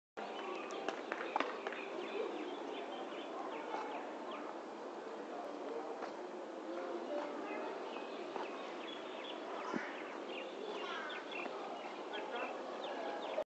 Floating Palace
Nice view of a floating palace nearby  the river